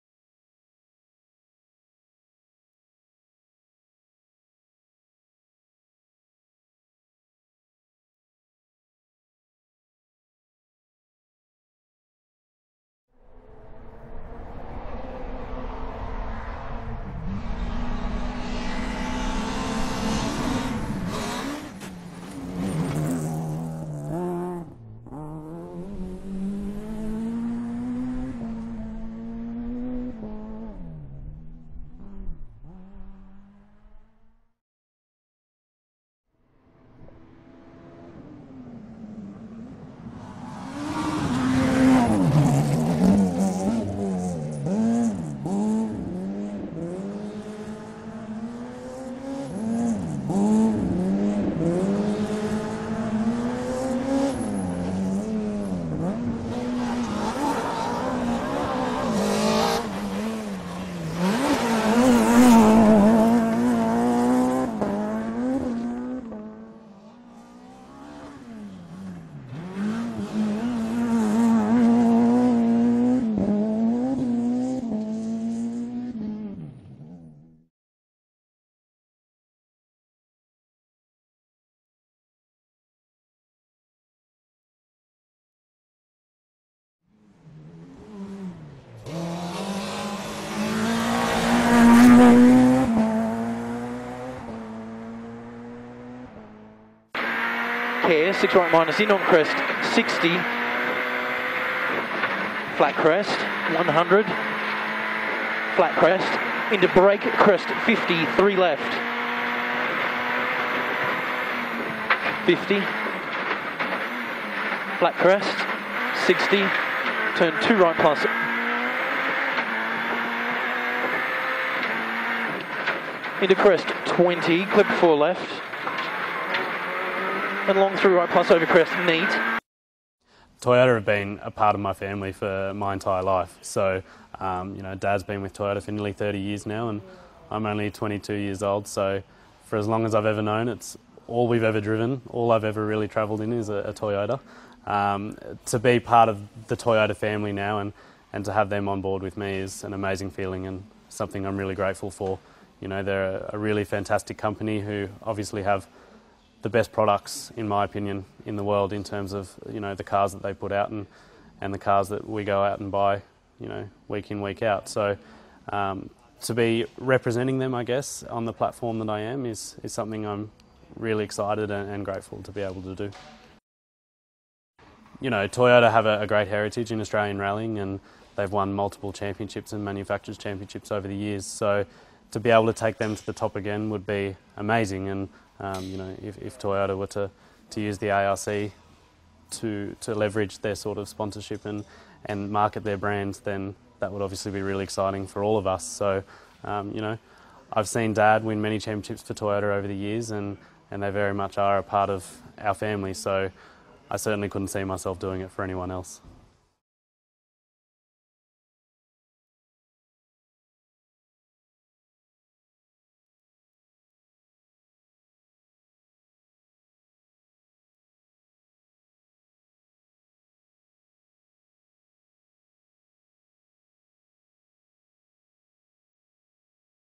Toyota Genuine Parts Corolla driver Harry Bates Victory in Canberra Puts Toyota's Bates Back On Top.
BATES VICTORIOUS IN RALLY OF CANBERRA

* Victory in Canberra puts Toyota's Bates back on top
* Bonus point for seven stage victories
* Rough roads and fluctuating weather prove testing